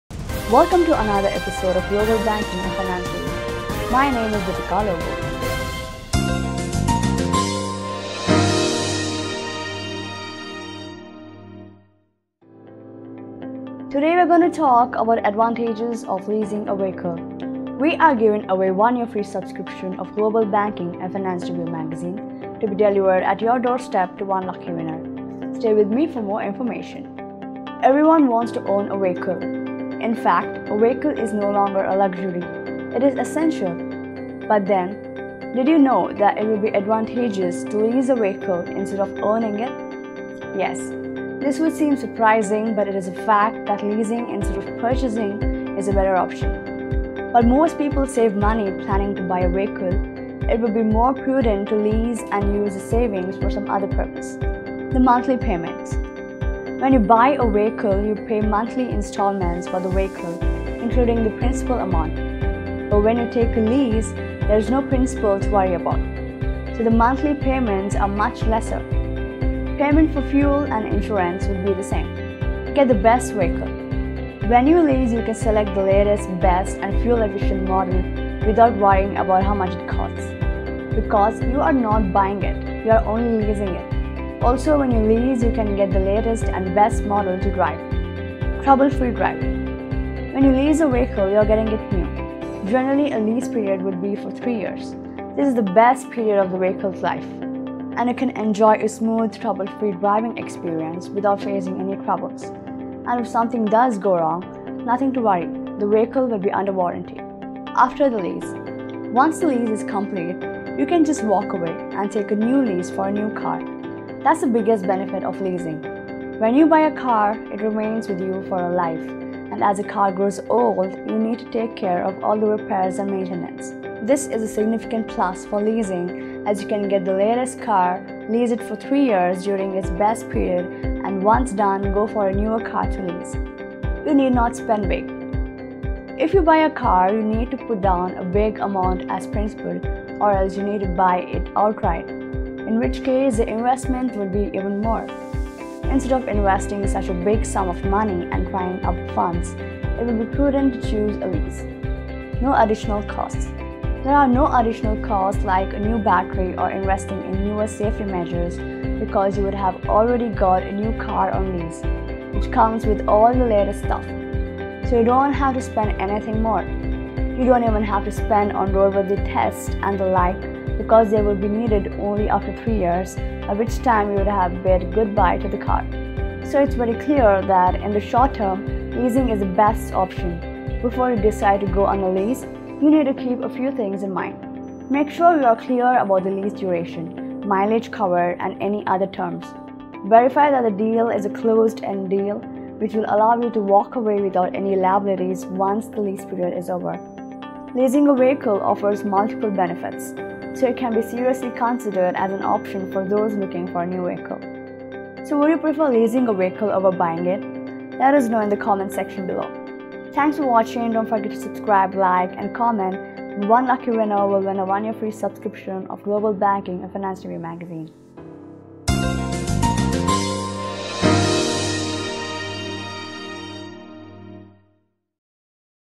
Advantages of Leasing a Vehicle | Is leasing better than Buying?
Everyone wants to own a vehicle. In fact, a vehicle is no longer a luxury, it is essential. But then, did you know that it would be advantageous to lease a vehicle instead of owning it? Yes, this would seem surprising, but it is a fact that leasing instead of purchasing is a better option.
While most people save money planning to buy a vehicle, it would be more prudent to lease and use the savings for some other purpose.